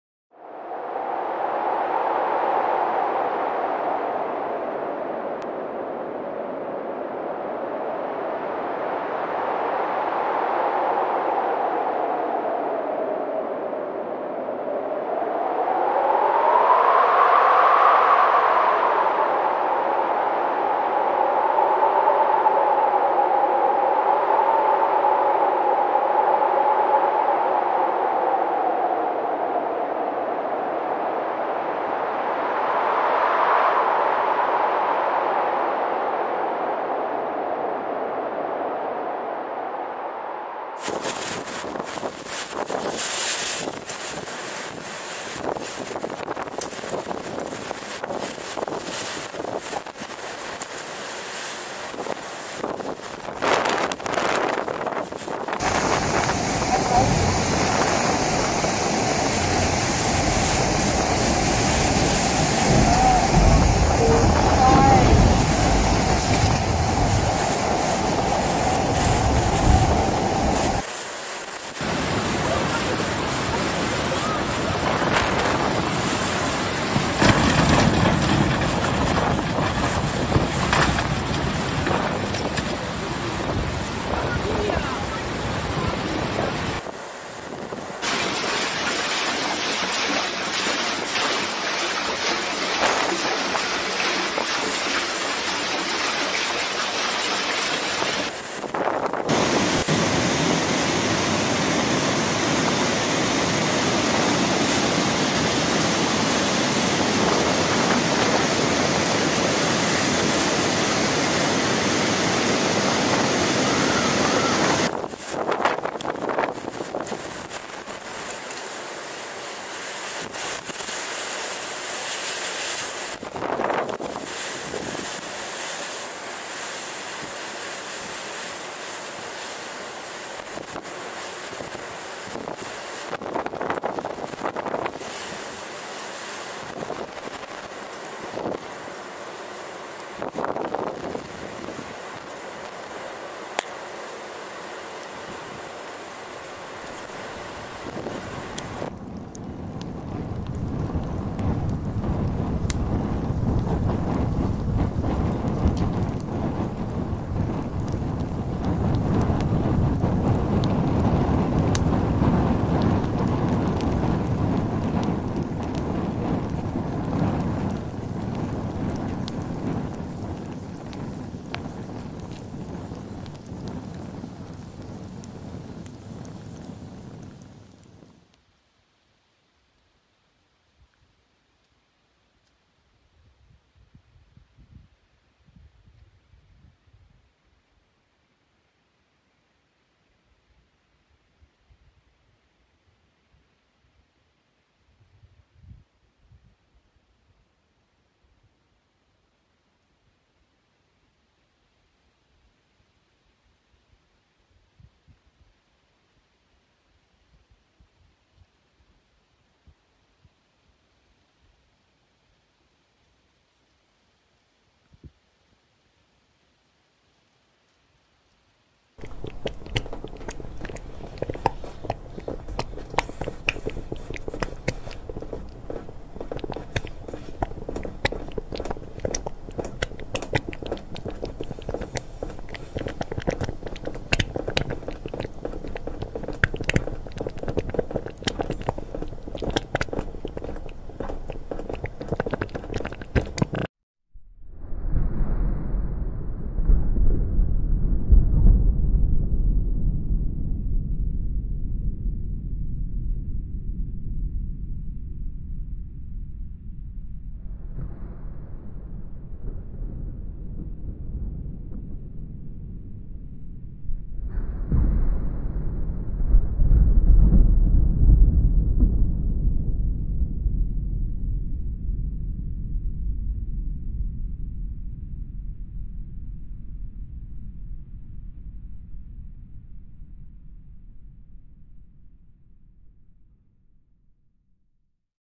The Beginning of Sorrows Scripture
The Bible predicted that there will be a time when the Earth will be filled with war, destruction, and natural disasters.

When we hear rumors of war, destruction, and natural disasters, we should not just ignore them as if it's nothing. We should know that these are the signs mentioned by the Bible during the end-times prophecies.